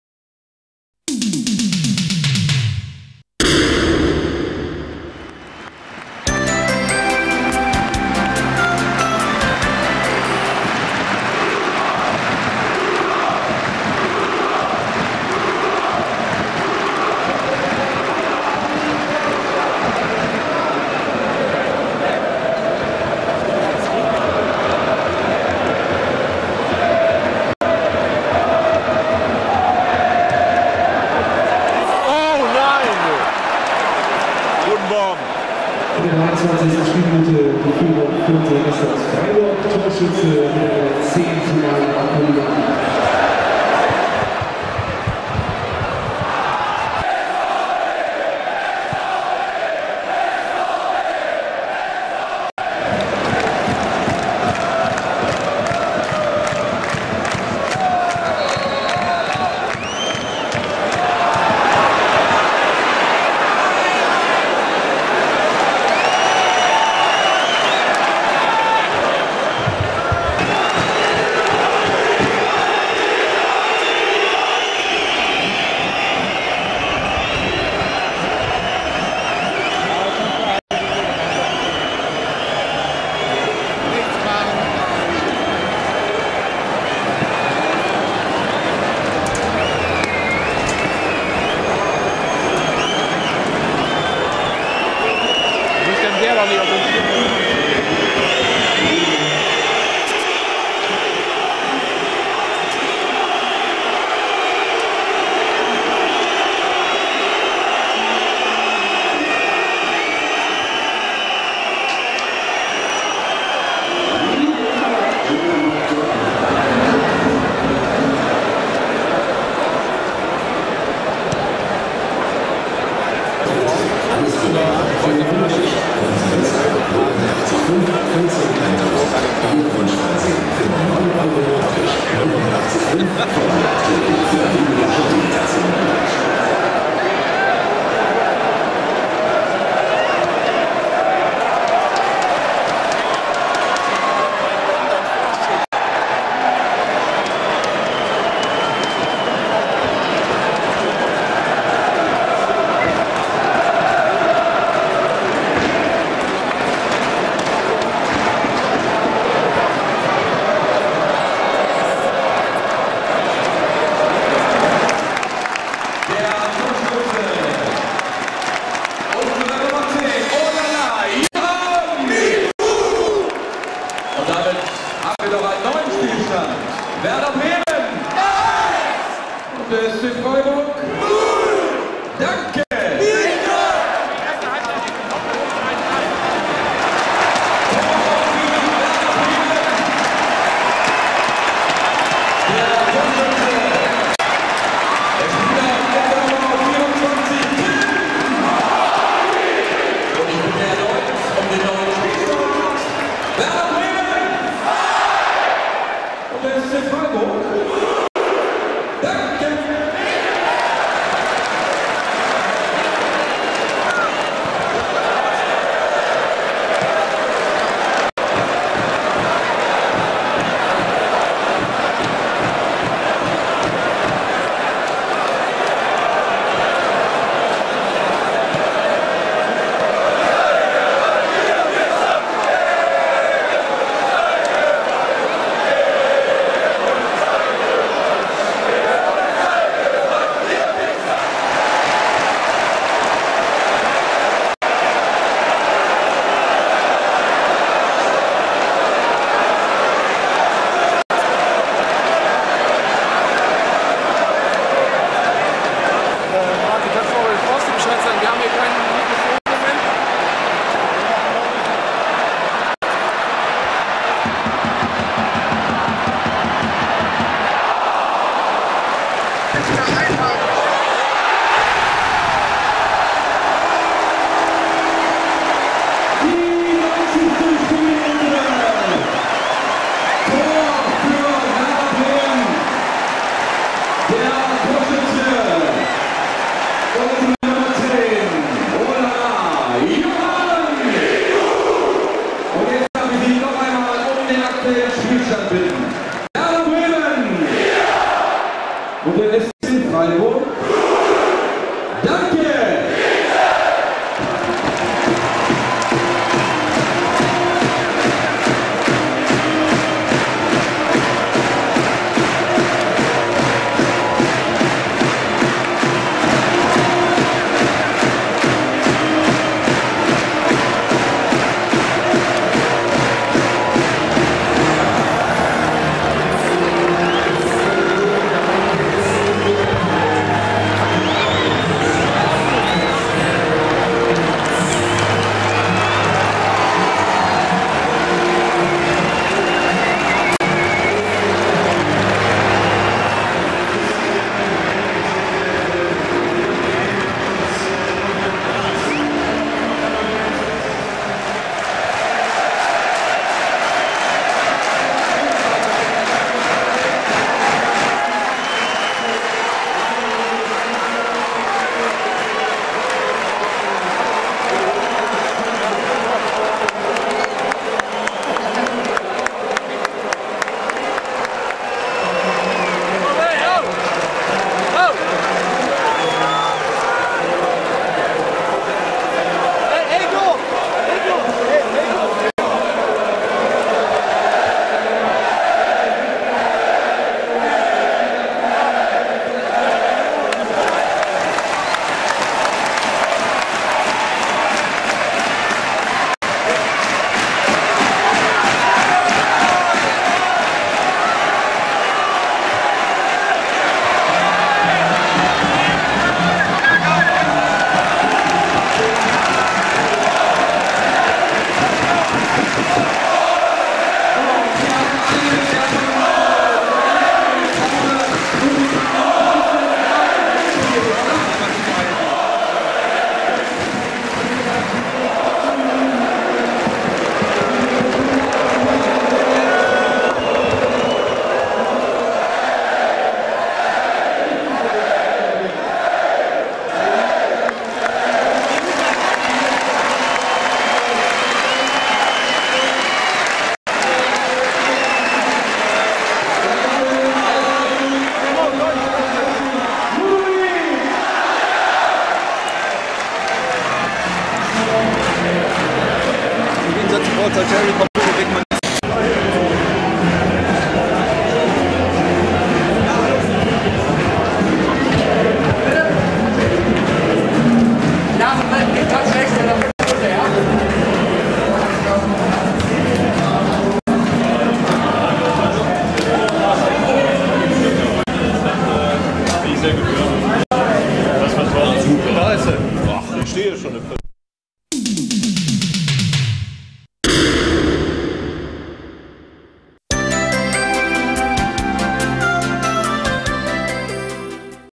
Werder Bremen - SC Freiburg Stadionbilder 2005 *Werder TV Hünniger 2005
Werder TV Original mit den Stadionbildern vom letzten Saisonspiel 2004/05 Werder Bremen - SC Freiburg im Weserstadion